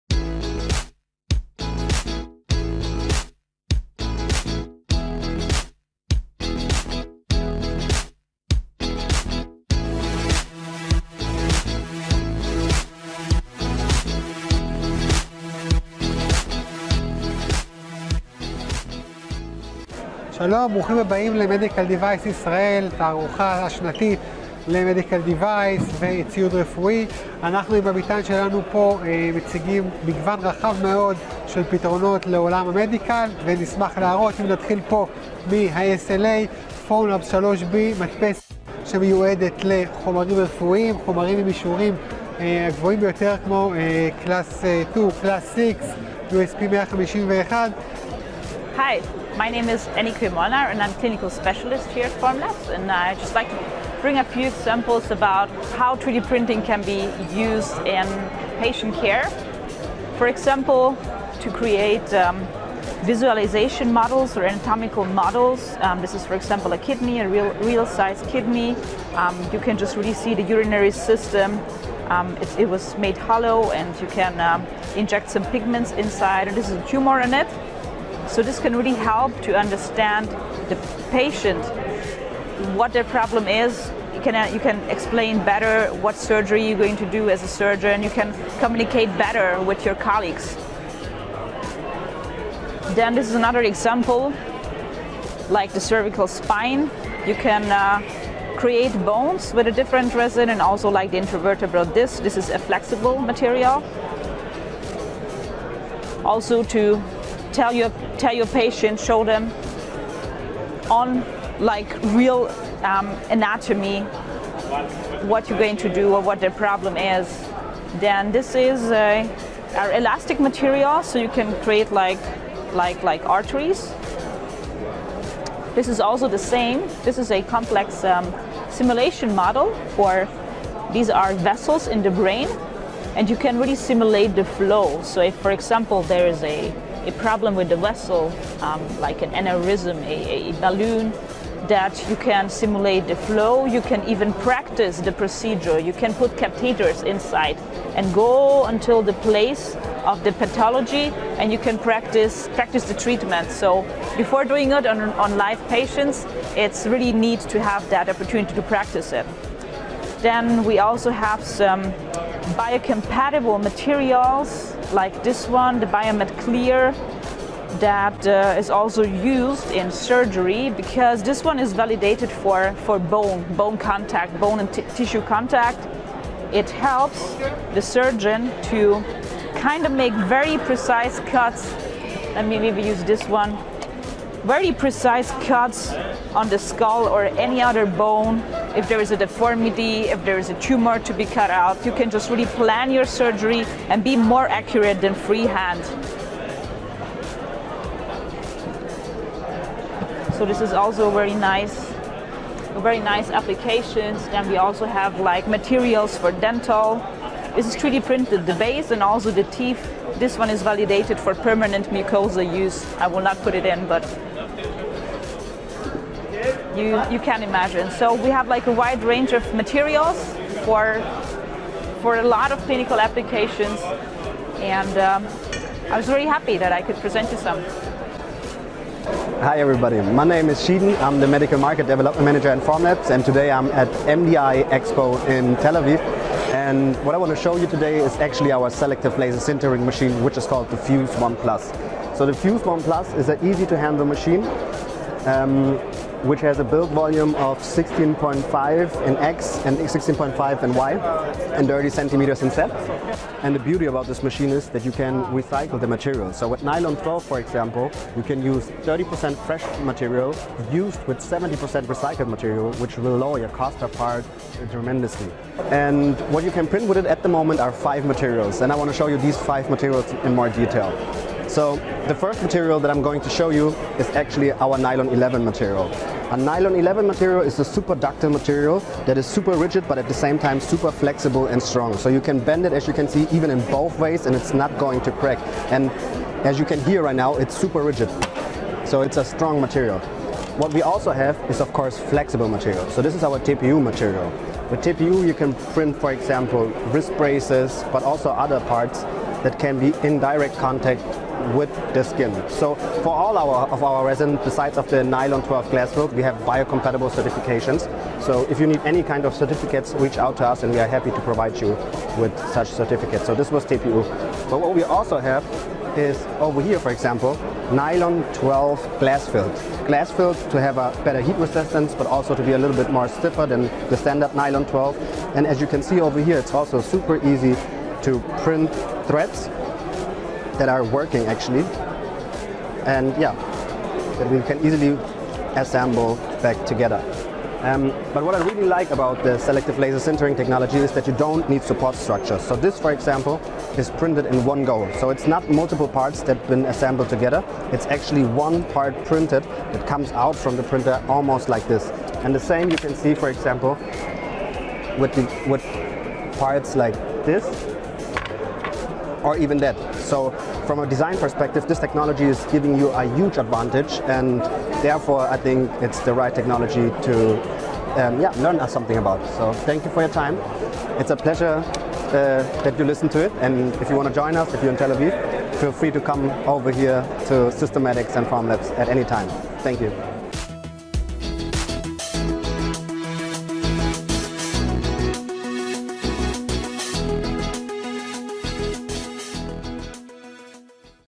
חידושים בהדפסת תלת ממד לייצור ציוד רפואי
הדפסה תלת ממדית מוטמעת באופן נרחב בתהליכי פיתוח מיכשור רפואי ובמרכזים רפואיים. באמצעותה ניתן לחסוך זמן פיתוח יקר, להפחית עלויות בצורה ניכרת ולייצר עזרים רפואיים מותאמים אישית.
נציגים של היצרניות HP וFormlabs- הצטרפו אלינו לכנס MDI לציוד רפואי והציגו את הפתרונות והטכנולוגיות החדשות בתחום הציוד הרפואי.